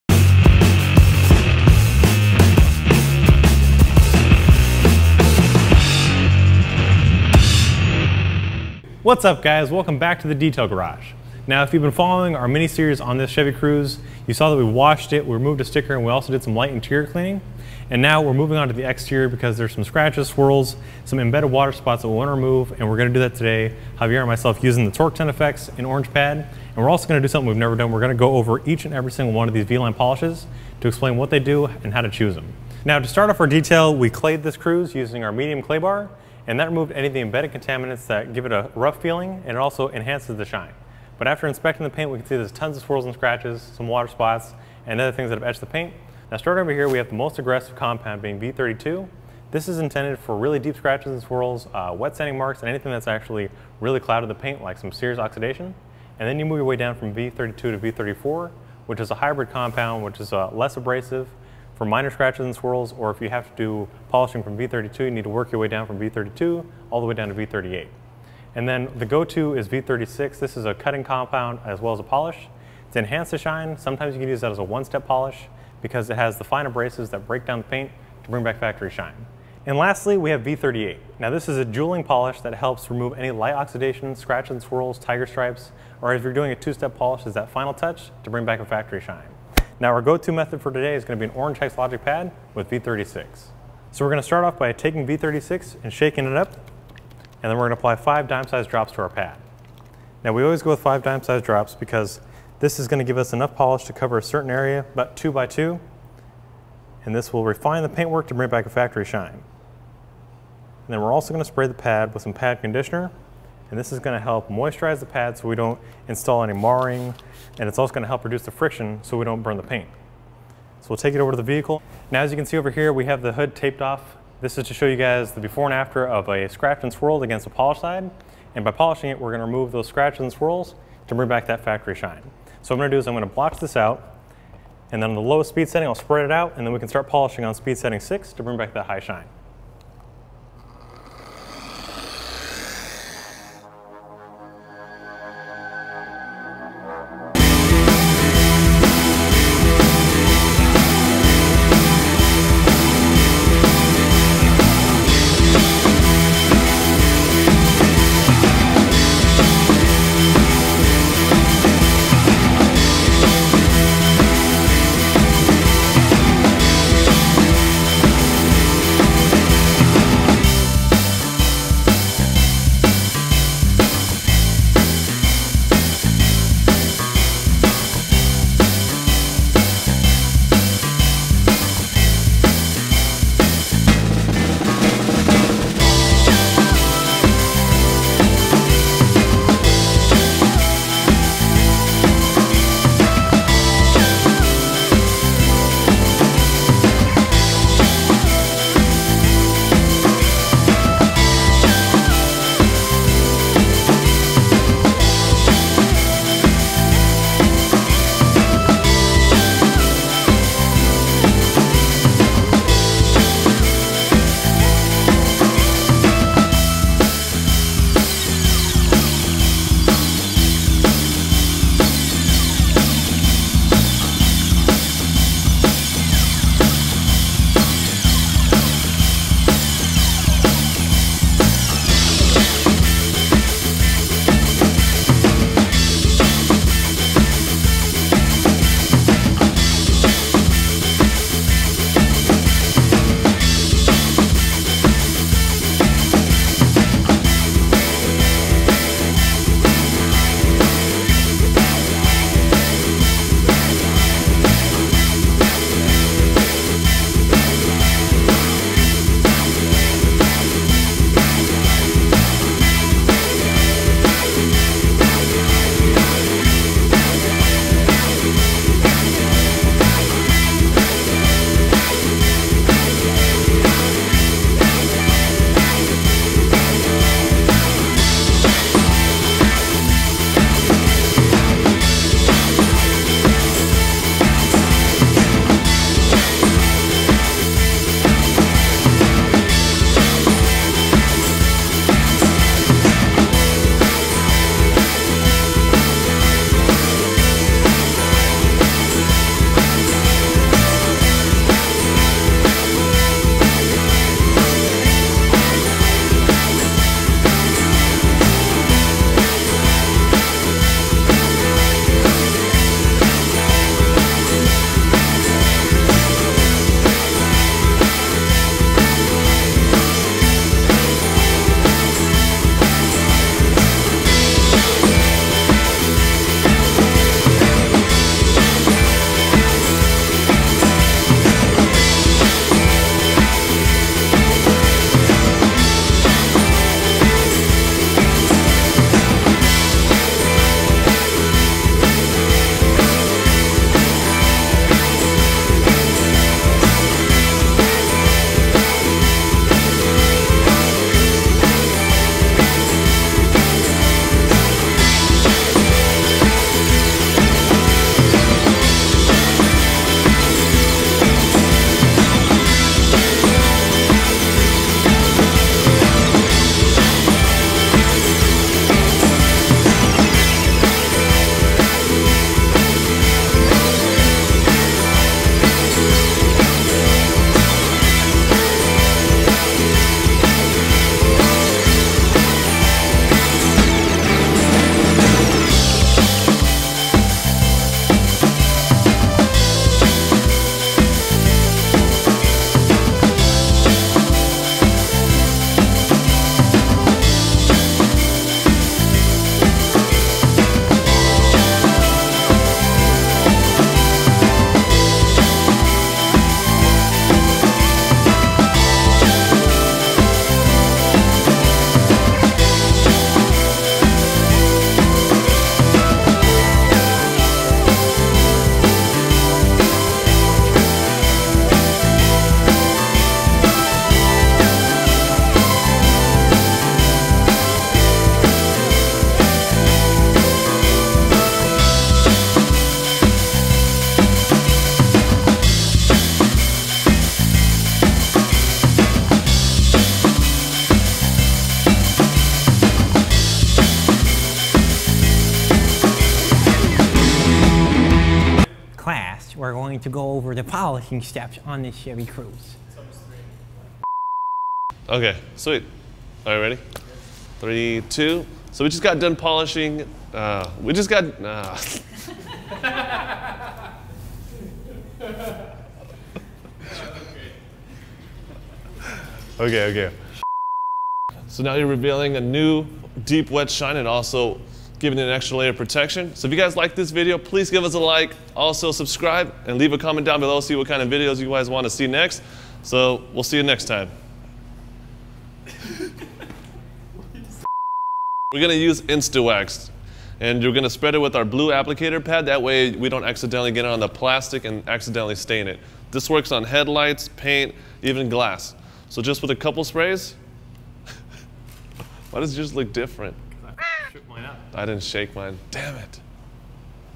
How To Make A Used Car Look New! - Chemical Guys Car Care
Chemical Guys demonstrate how to correctly remove scratches and swirls from your car's paintwork. Chemical Guys also demonstrate how to correctly use a polisher. In addition, Chemical Guys go over the differences of the V-line polishes, and explain which polish is best for each situation. Lastly, Chemical Guys explain how to apply a spray wax to paint, and how to remove it.

The audience has been watching intently, but they still have questions. What’s the difference between the V-Line polishes? How do I use them to get rid of those pesky scratches? HELP US! Class is in session, and Professor Nick is prepared to give his keynote speech on the finer points of polishing; pay attention and take extensive notes because there’s a lot of ground to cover. In the last video, we showed some DIY techniques on how to clean and clay bar your vehicle. The canvas we’re working on is a Chevy Cruze with a metallic flake paint job. In order to restore the paint on this vehicle, it’s important to note how to utilize the V-line of polishes.

V32 and V34 are compounds containing heavy abrasives that remove deep scratches, and restore the shiny deep luster we all desire from our car’s paint. V36 is a hybrid compound that breaks down into a fine polish. V38 is a jeweling polish that refines the paint in order to remove buffer trails. The tips you see here are something you can do yourself, and achieve results like the professionals of the automotive industry. Before you begin polishing, you must give the car a thorough car wash, which was done in the previous video. Dab the appropriate compound on your pad in five dime-sized drops, apply the compound to the paint in a 2x2 area, place your dual-action polisher on the paint, set it to speed setting one, and spread the polish on the paint.

Protecting your paint after polishing is very crucial. That's why Nick and Javier spray the paint exterior with Instawax. Just spray Instawax on the paint, spread it around with a microfiber applicator pad, and buff off after five minutess. With these how-to auto detailing tips and tricks, it's easy to get a deep glossy shine. Remember to use your microfiber towels  wisely, and don't forget to pick up the best car wax from Chemical Guys.